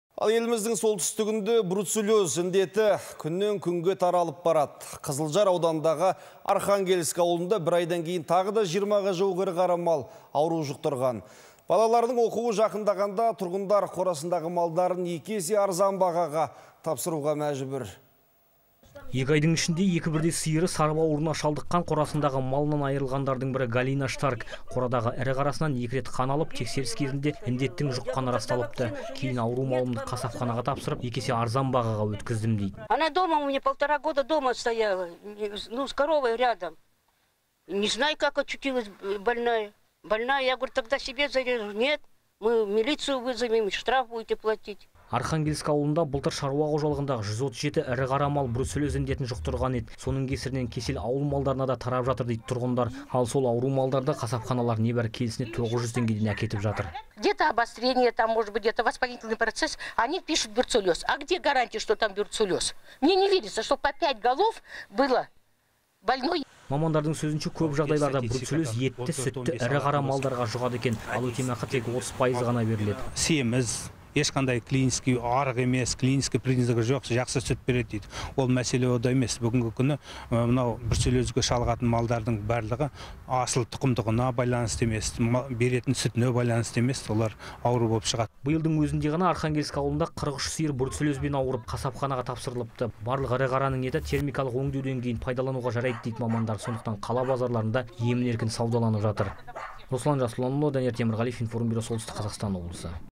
Солтүстік Қазақстанда бруцеллез індеті күннен-күнге таралып барады
Еліміздің солтүстігінде бруцеллез індеті күннен-күнге таралып барады. Қызылжар ауданындағы Архангельск ауылында бір айдан кейін тағы да жиырмаға жуық ірі қара мал, ауру жұқтырған. Балаларының оқуы жағындағанда, тұрғындар қорасындағы малдарын екі есе арзан бағаға тапсыруға мәжбүр.